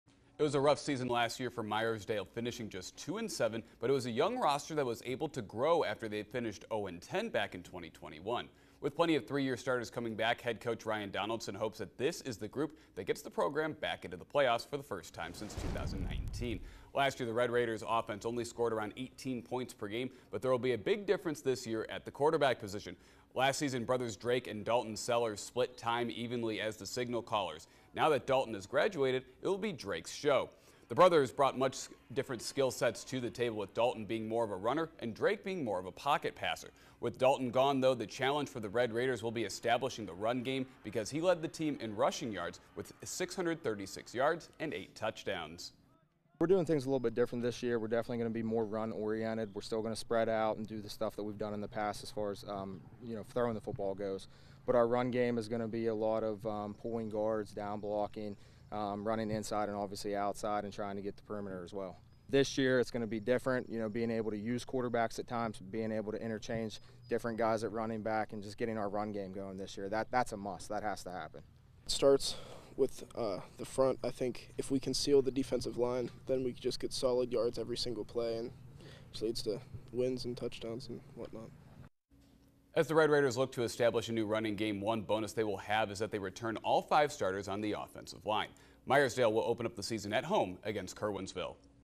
Sportsbeat Preview: Meyersdale
Meyersdale looks to improve upon a 2-7 record last season and establish a new rushing attack.